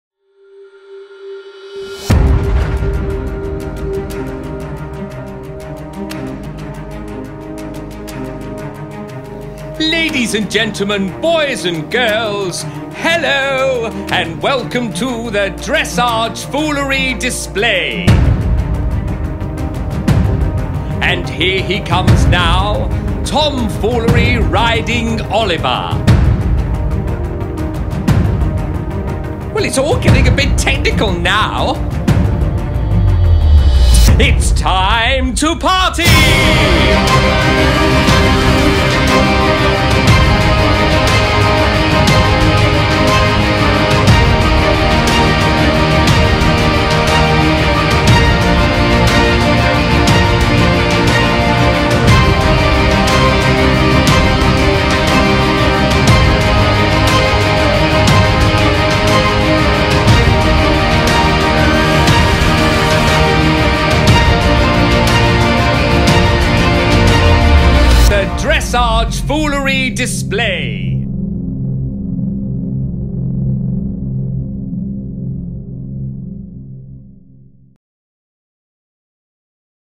Tomfoolery in the DressageFoolery Display
Tomfoolery joined forces with popular riding instructor and rider Darren Woods, to put together this hilarious display of horsemanship!

Can Tomfoolery really ride like that! Dressage is the art of dancing and look elegant, does Tomfoolery have what it takes!

See more brilliant Tomfoolery  videos on his Channel Tomfoolery TV

See Tomfoolery look after the animals at Tilgate park as part of Radio Foolery: